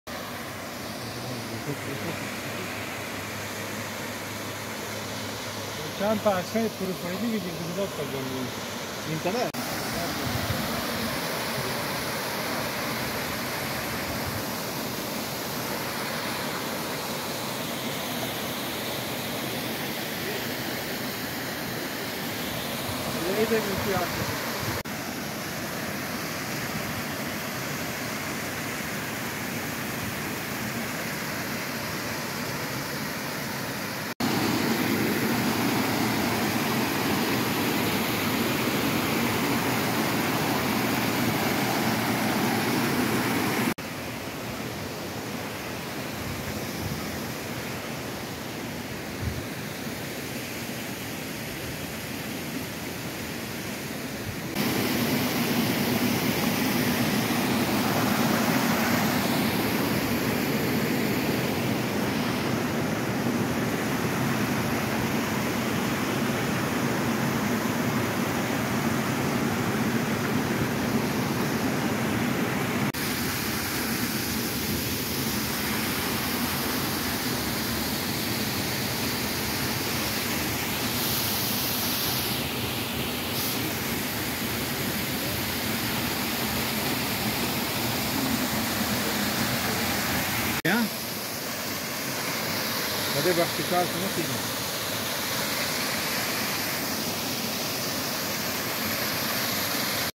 Triberg waterfalls Germany
The Triberg Waterfalls are waterfalls near Triberg in the Black Forest in Baden-Württemberg. With a descent of 163 m, it is one of the highest waterfalls in Germany and a landmark in the Black Forest region.